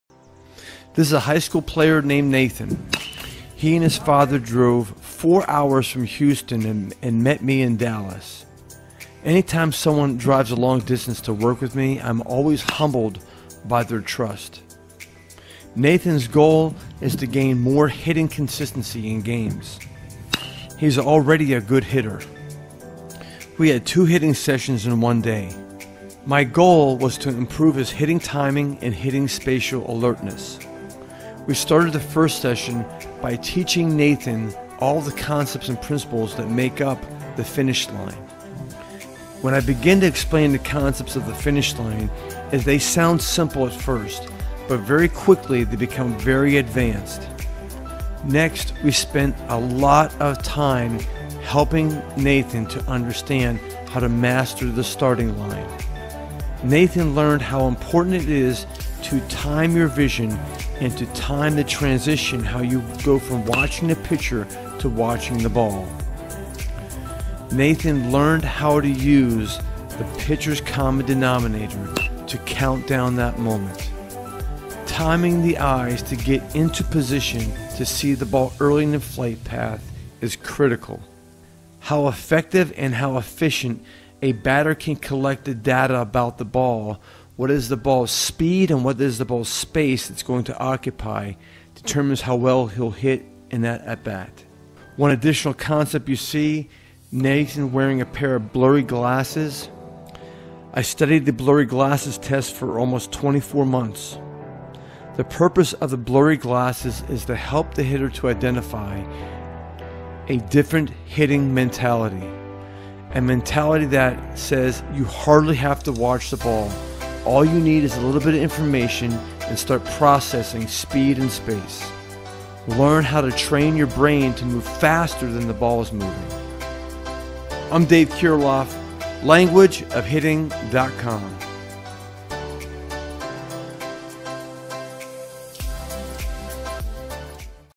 How to Hit Better: They Drove 8 Hours, Working Hitting Timing & Spatial Alertness!
I am humbled every time a player makes a long journey to come and work with us.  This was the story with Nathan and his father.  They live near Houston TX.  They drove to Dallas TX to learn about the delicate details of Hitting Timing and Hitting Spatial Alertness.  From start to finish, 8 hours of driving time.
(SEE VIDEO)

Dave Kirilloff online hitting coach.
Alex Kirilloff Minnesota Twins.